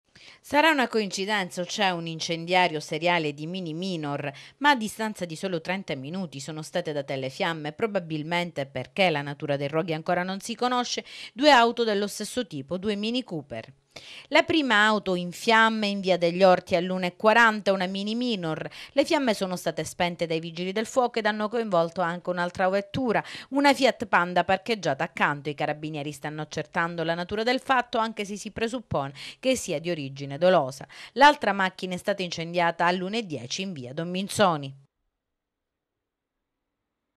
DUE AUTO IN FIAMME CITTA’
DUE AUTO IN  FIAMME   IN VIA DEGLI ORTI  E IN VIA DON MINZONI, LA COINCIDENZA ENTRAME MINI COOPER